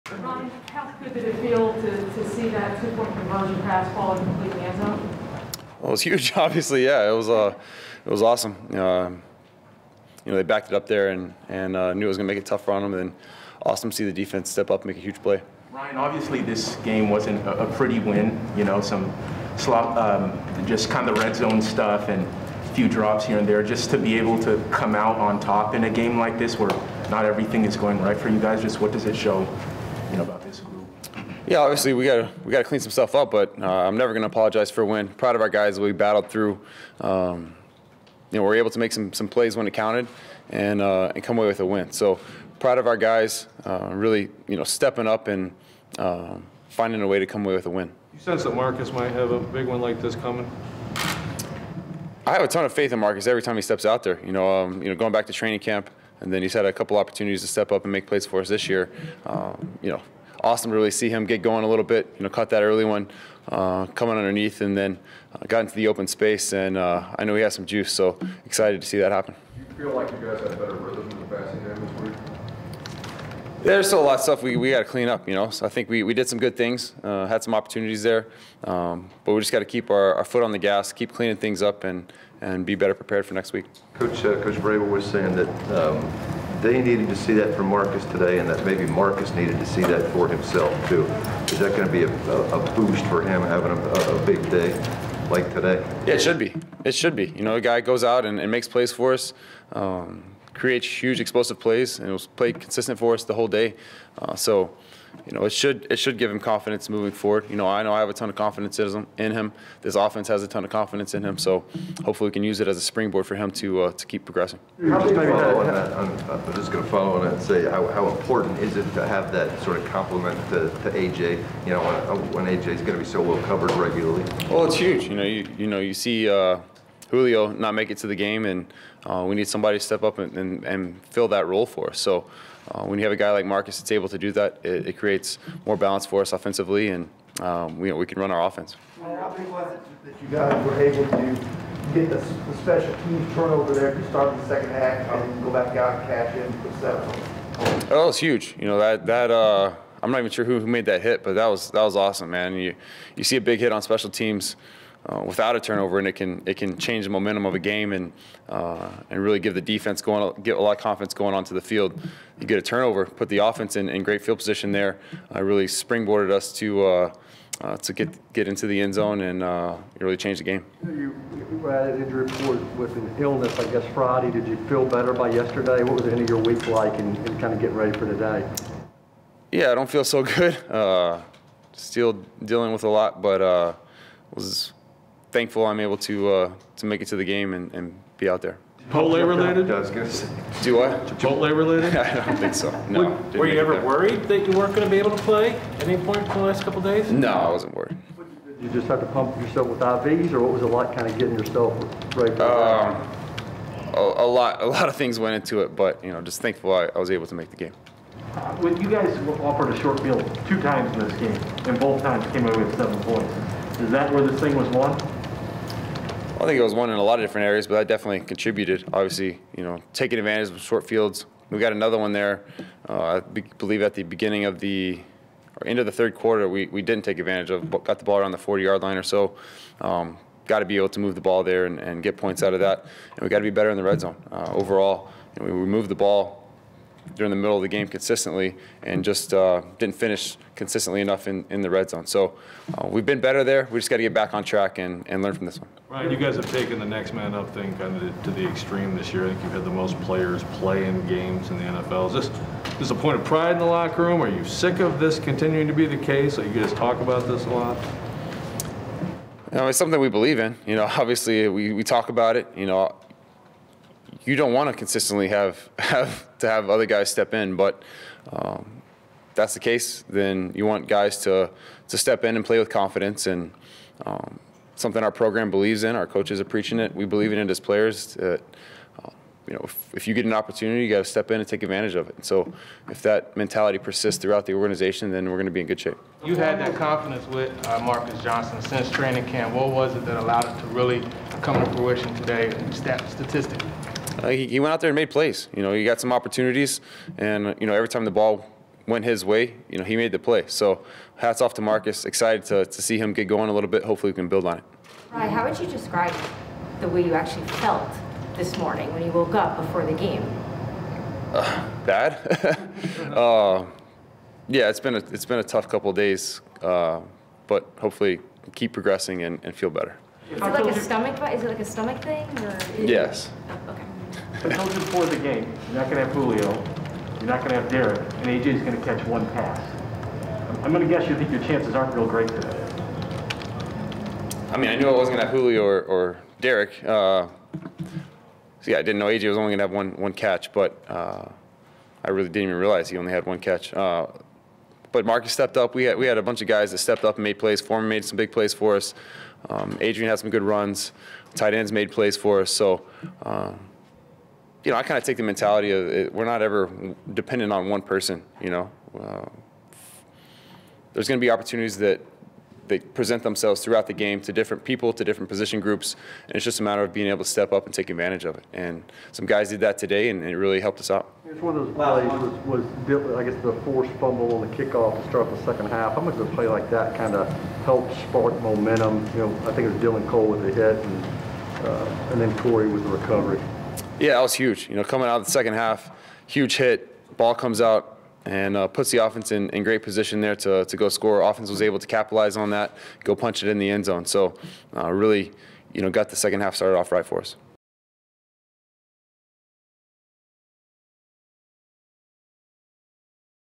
We Had a Bunch of Guys That Stepped Up | Ryan Tannehill Player Interview
Titans QB Ryan Tannehill addresses reporters following Tennessee's 23-21 victory over the New Orleans Saints on Sunday at Nissan Stadium.